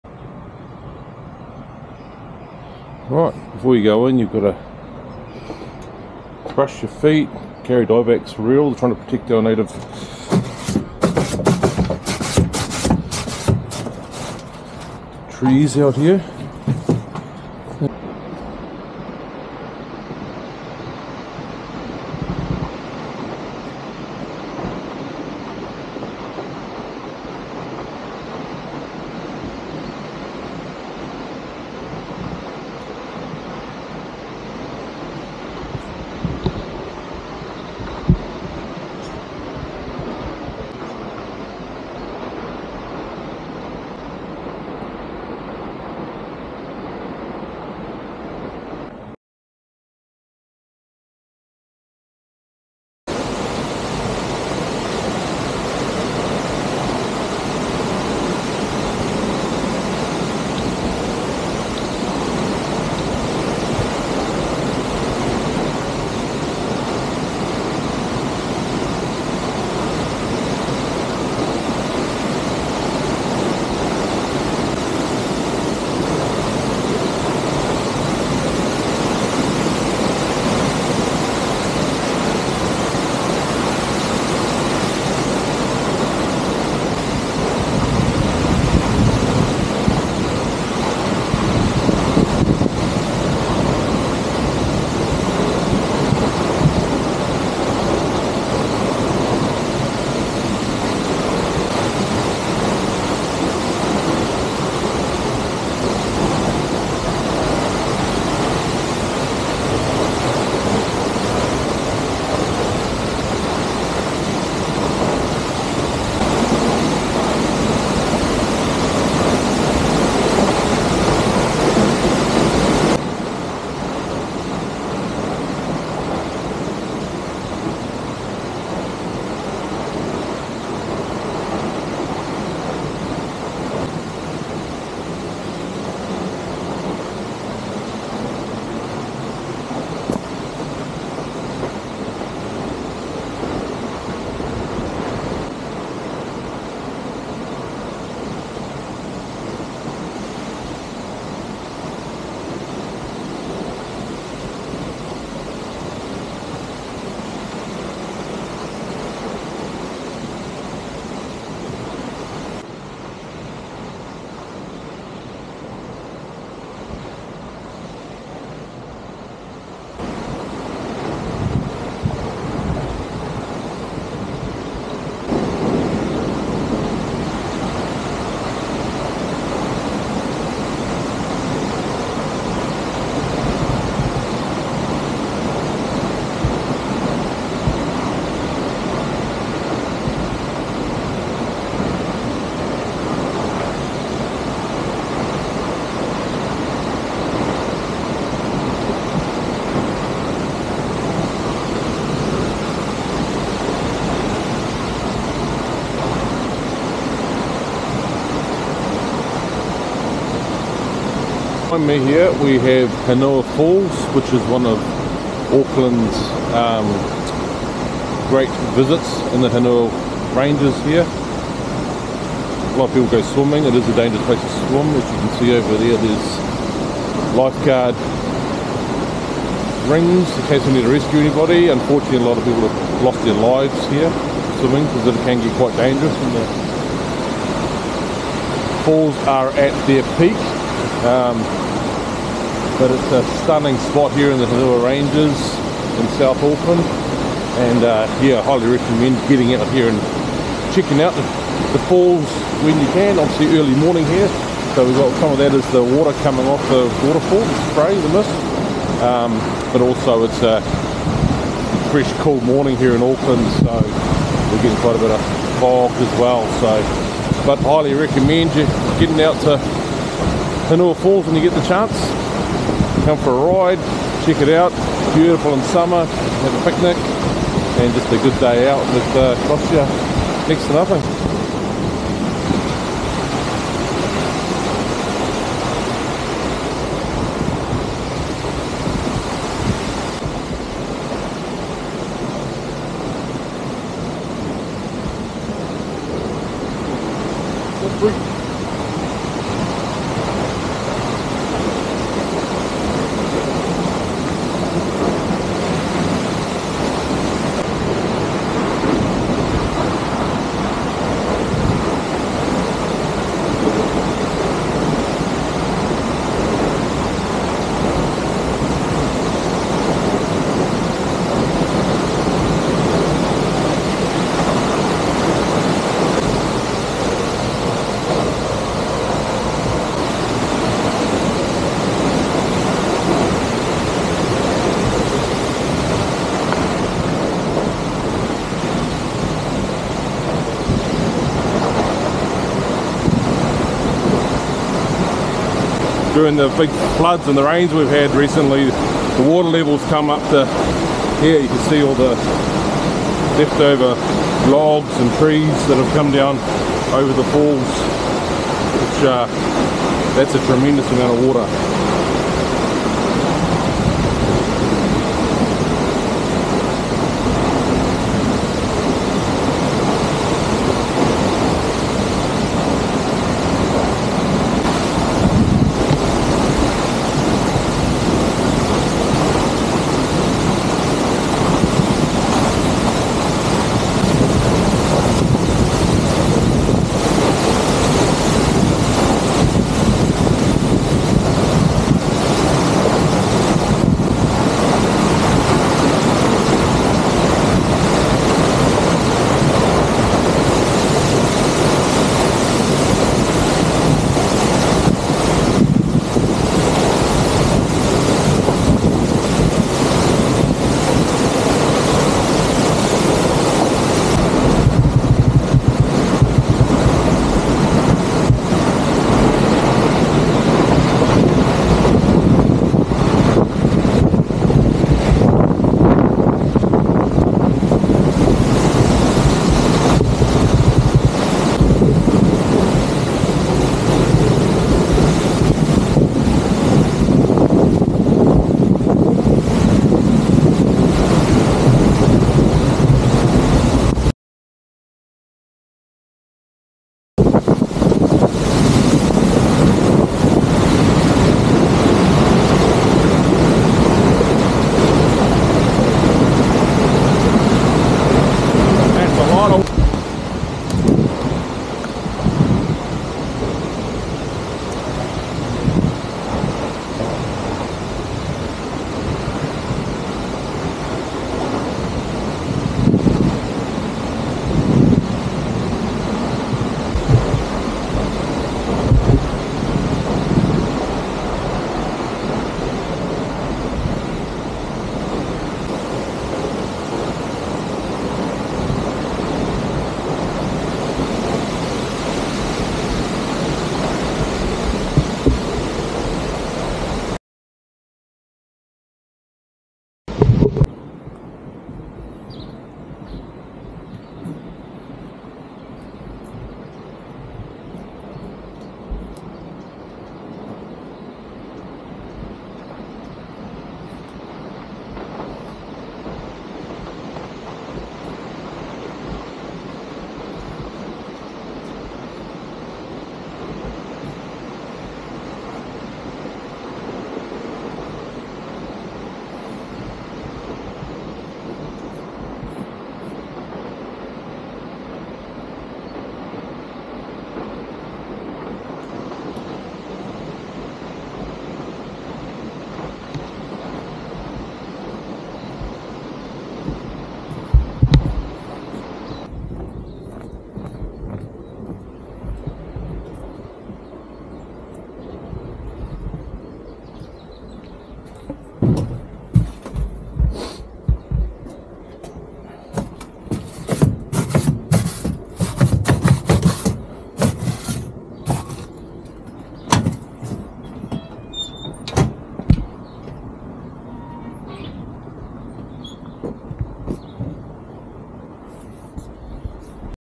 Early Sunday morning ride to Hunua Falls
Short video of walking up to Auckland’s Hunua falls on a crisp Sunday morning.